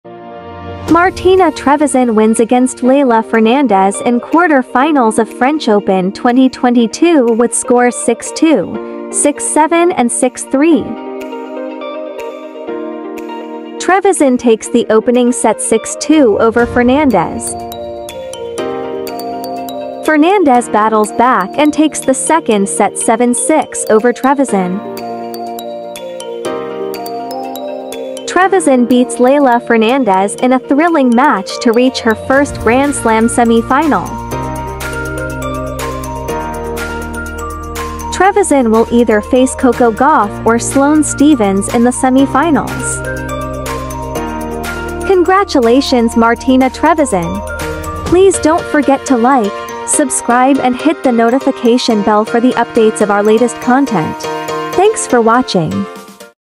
LEYLAH FERNANDEZ WAS DEFEATED BY MARTINA TREVISAN IN THE QUARTERFINAL | FRENCH OPEN 2022 | RG22
Photos are credited to the owners